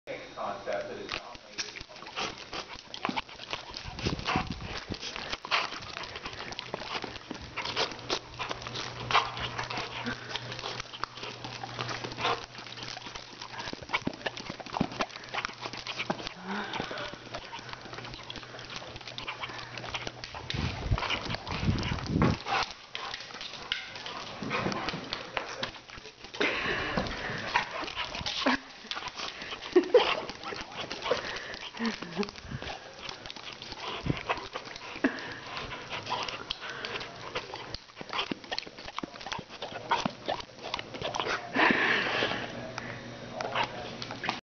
Maximus the Magnificent Drinking Water..from a spray bottle!!!
Max, our 11 month old Rottweiler puppy, loves to drink water from just about anything, including a spray bottle!  HILARIOUS!!!